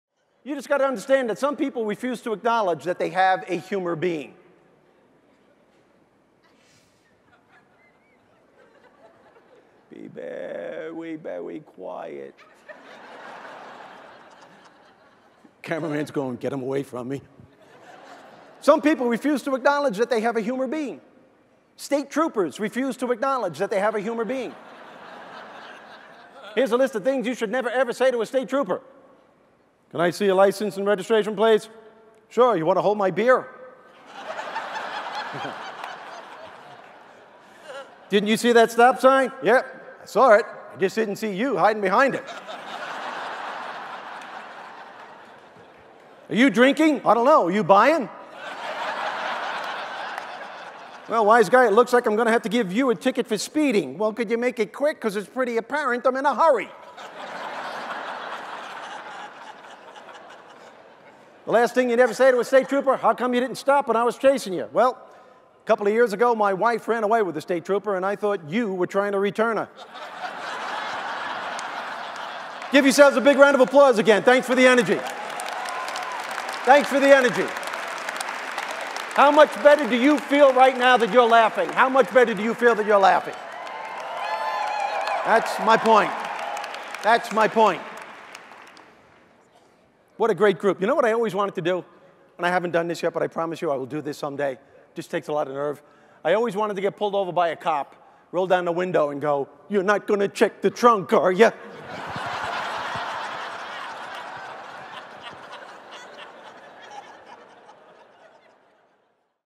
Funny Business Speaker Steve Rizzo: Some People Don't Have a Sense of Humor
Funny Motivational Speaker Steve Rizzo captivates an audience of 8000 using his own brand of comedy to demonstrate how there are some people who don't have a sense of humor. State Troopers don't have a sense of humor. Try this at home, great dinner table talk.